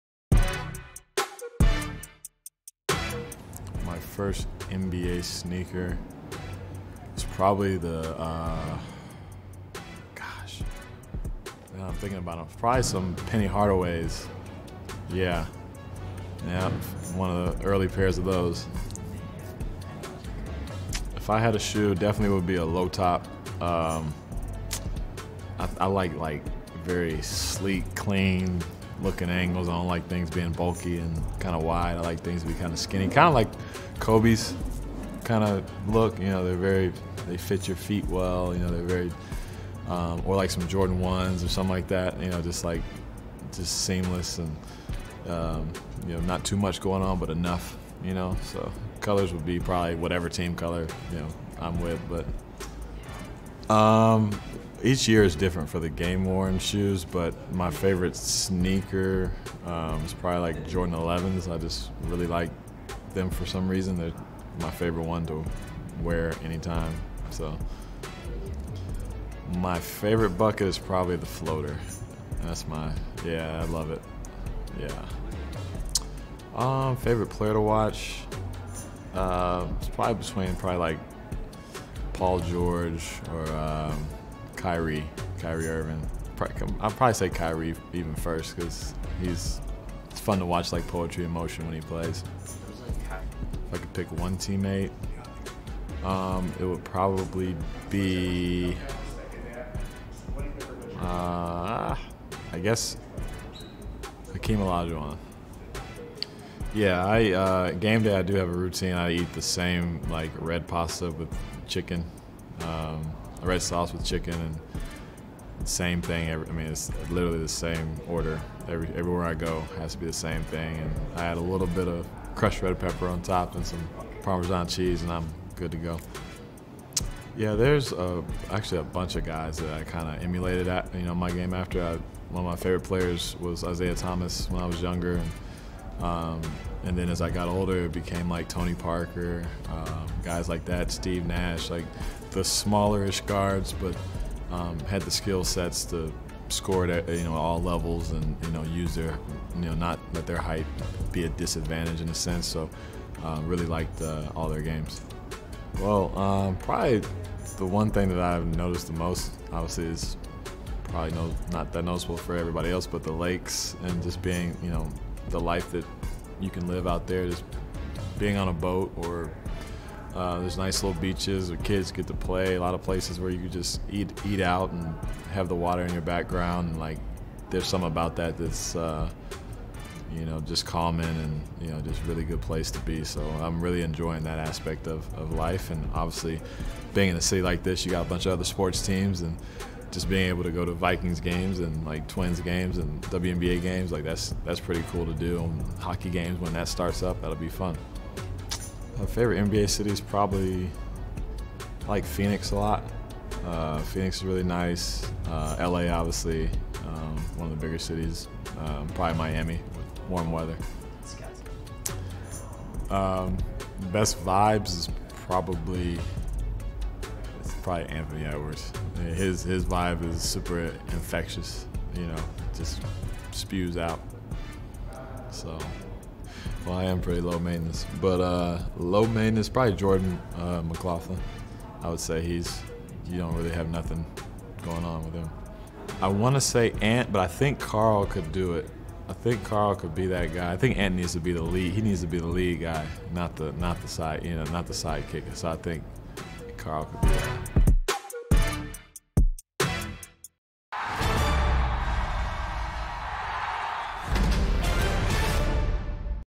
Get to know: Wolves guard Mike Conley
Get to know veteran guard Mike Conley before the Timberwolves host the Lakers at 8 p.m. on Bally Sports North.